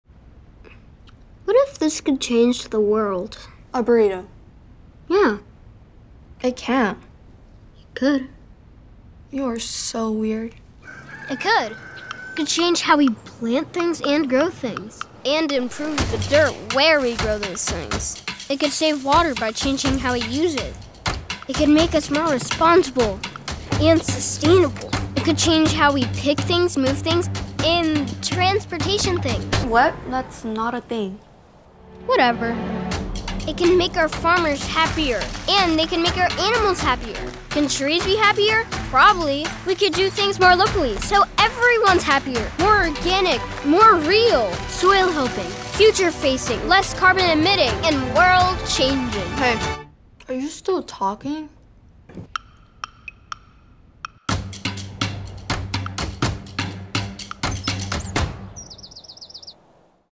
Chipotle | Can a Burrito Change the World? [Full Version]
What if a burrito could change the world?

It could change how we plant things, water things, grow things, pick things, move things...it could change all the things. It could make our famers happier. More organic. More real. More soil-helping, less carbon emitting, and world changing.

Learn more about the sustainable impact Chipotle is making through a commitment to Food with Integrity

Discover the difference Chipotle is making to help the next generation of farmers through a commitment of $5-million over the next 5 years